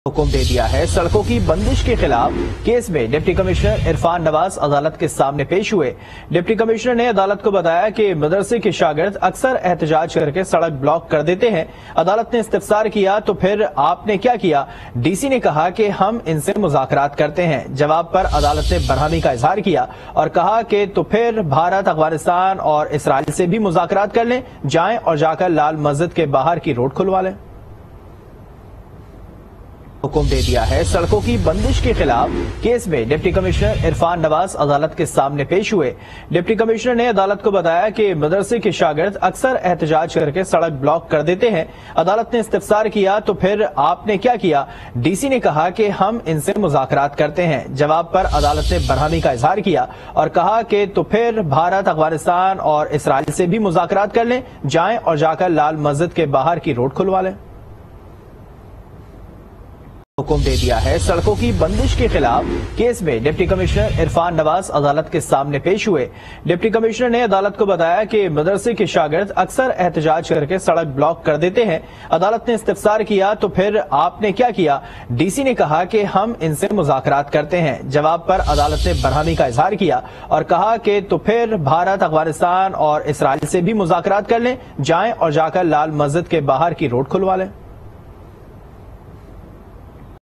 Islamabad high court issued important order regarding road blocking case | Aaj News
Aaj TV Official:
shorturl.at/iBDNX